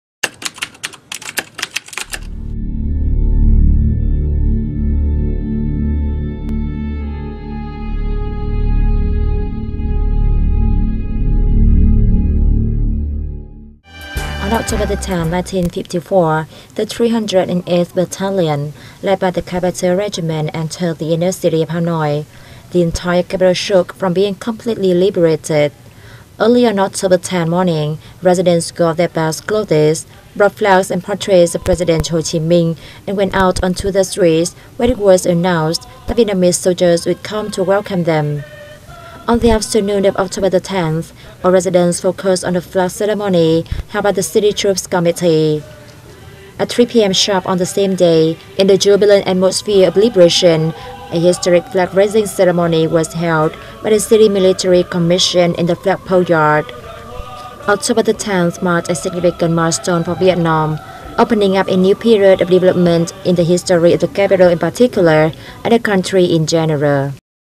Historic moments of October 1954 in Hanoi
Those living in Vietnam’s capital Hanoi will be clearly feeling two special things at this time: the poetic beauty of the autumn weather and the joyful and exciting atmosphere as the city celebrates its Liberation Day, October 10, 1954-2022.